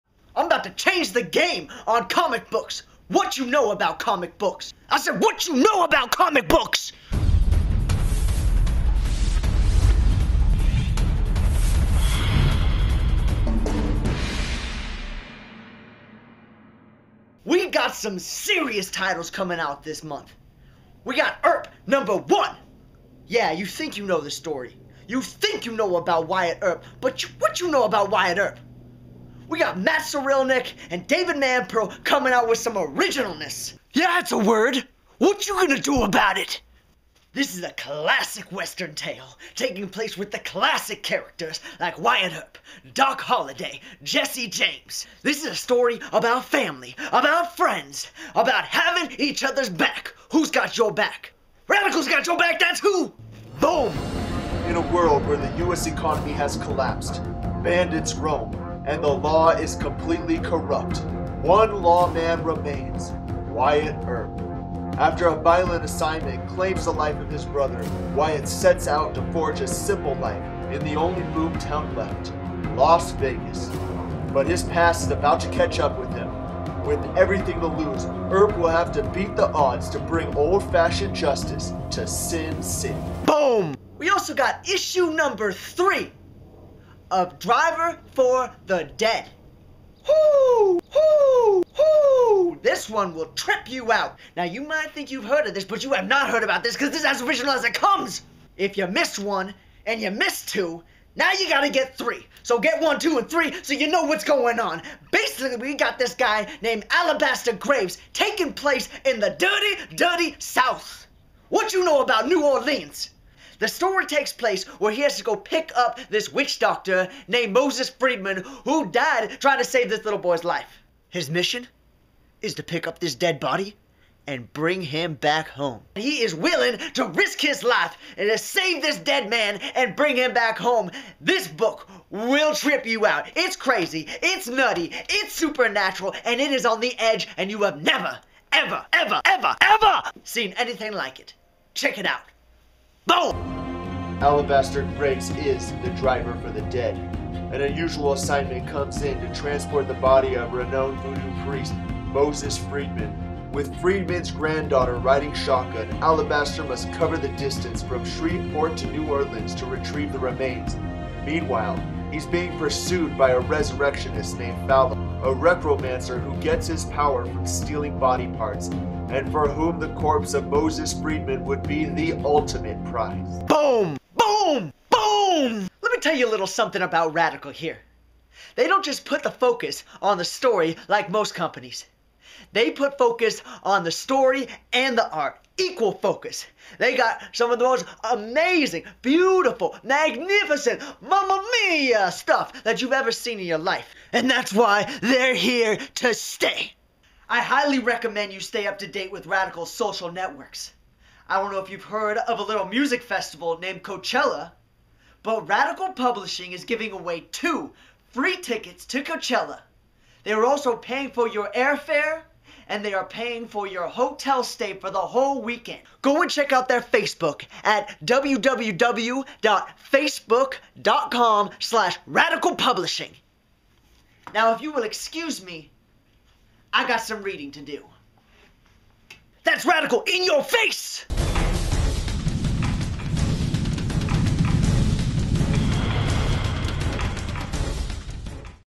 R News episode #1: EARP#1 and Driver for the Dead #3
Watch our debut R News segment to get the latest on the happenings in the Radical universe!

For details on how to enter the COACHELLA CONTEST mentioned in this segment go to our contest page on Facebook

EARP-
Creators: Matt Cirulnick, David Manpearl
Writer: M. Zachary Sherman, Matt Cirulnick
Artists: Mack Chater, Martin Montiel
Colorist: Kyushik Shin

Driver for the Dead-
Writer: John Heffernan
Artist: Leonardo Manco
Colorist: Kinsun Loh & Jerry Choo